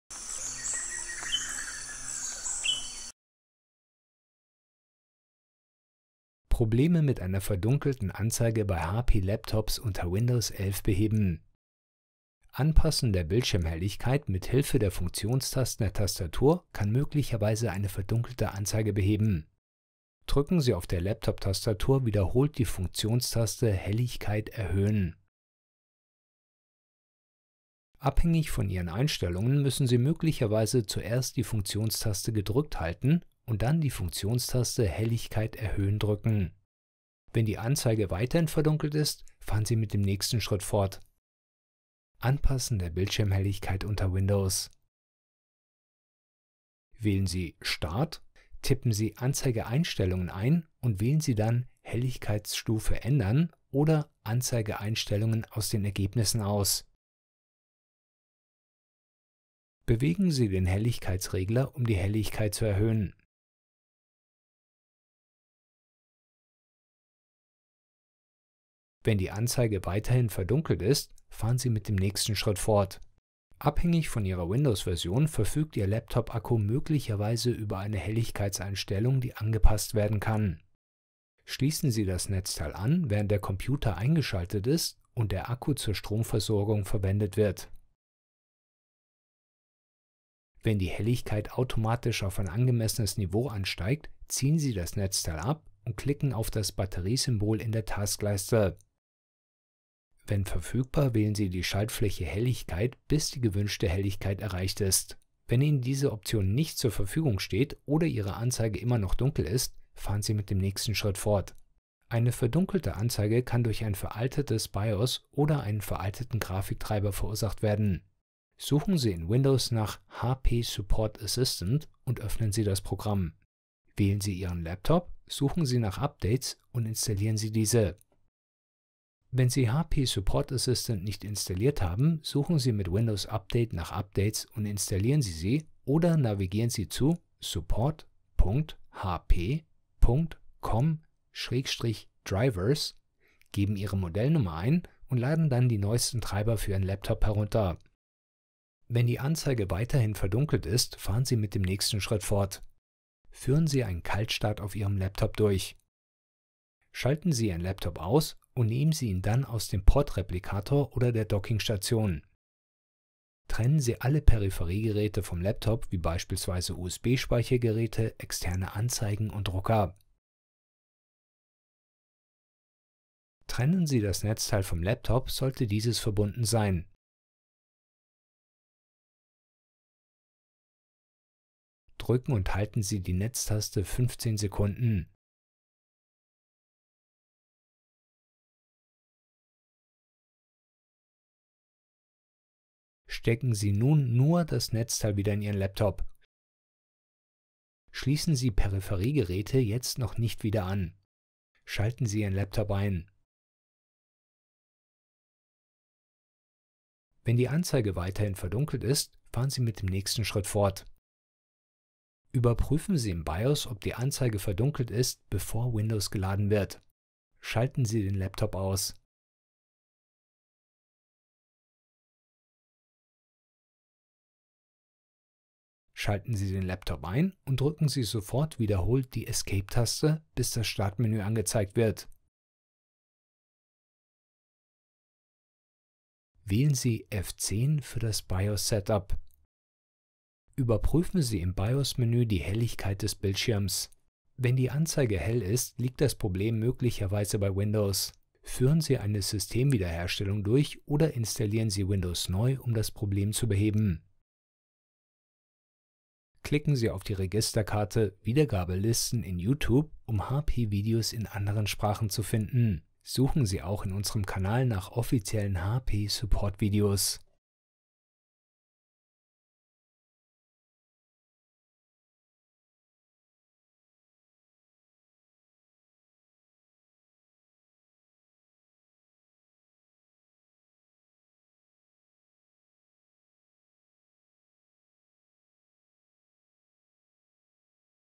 Problem mit einer verdunkelten Anzeige bei HP Laptops unter Windows 11 beheben | HP Support
Hier erfahren Sie, wie Sie ein Problem mit einer verdunkelten Anzeige bei HP Laptops unter Windows 11 beheben.